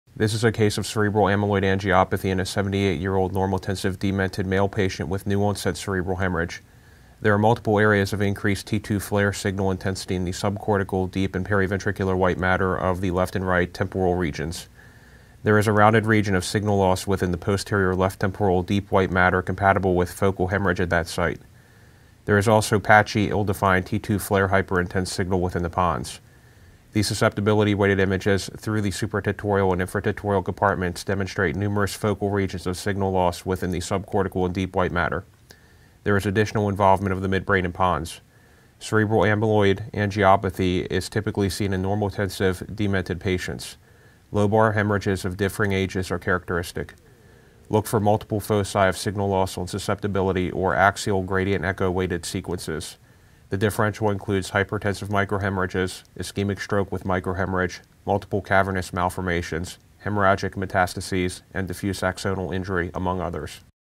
Cerebral Amyloid Angiopathy
78-year-old normotensive demented male patient with new onset cerebral hemorrhage. Multiple areas of increased T2/FLAIR signal intensity in the subcortical, deep, and periventricular white matter of the left and right temporal lobes including a rounded region of signal loss within the posterior left temporal deep white matter compatible with a focal hemorrhage at that site. There is patchy ill-defined T2/FLAIR hyperintense signal within the pons. Numerous focal regions of signal loss within the subcortical and deep white matter of the supratentorial compartment on the susceptibility weighted images. This is a case of cerebral amyloid angiopathy. Cerebral amyloid antipathy is typically seen in normotensive demented patients. Lobar hemorrhages of differing ages are characteristic.